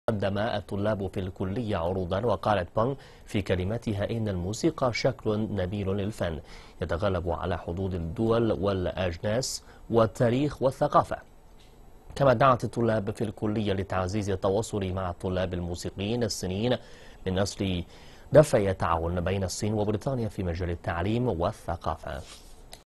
بنغ لي يوان تزور الكلية الملكية للموسيقى في لندن
تلبية لدعوة الملكة إليزابيث الثانية، سيقوم الرئيس الصيني شي جين بينغ
 بزيارة دولة إلى المملكة المتحدة، خلال الفترة من يوم 19 إلى يوم 23 أكتوبر الجاري.